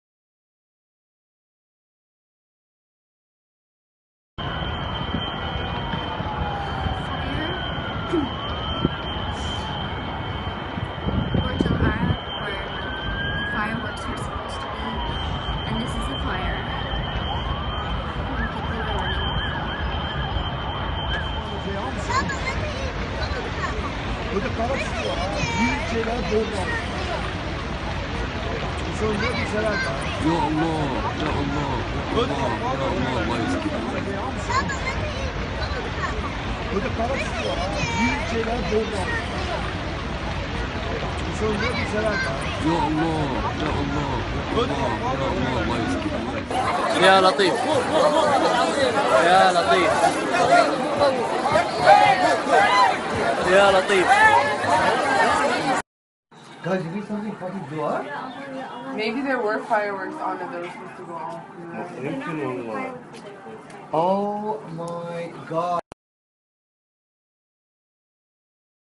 حريق ضخم يلتهم برج بدبي أثناء الاحتفال برأس السنة
حريق في أشهر الفنادق بدبي